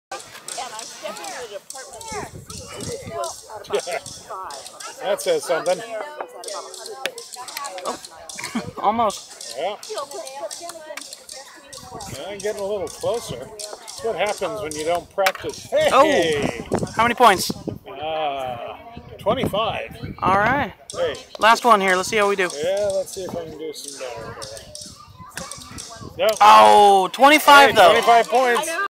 Mark Hall-Patton plays Tiddly Winks
Mark Hall-Patton playing Tiddly Winks at the 16th Annual Friends of the Price House ice cream social in Pismo Beach, CA.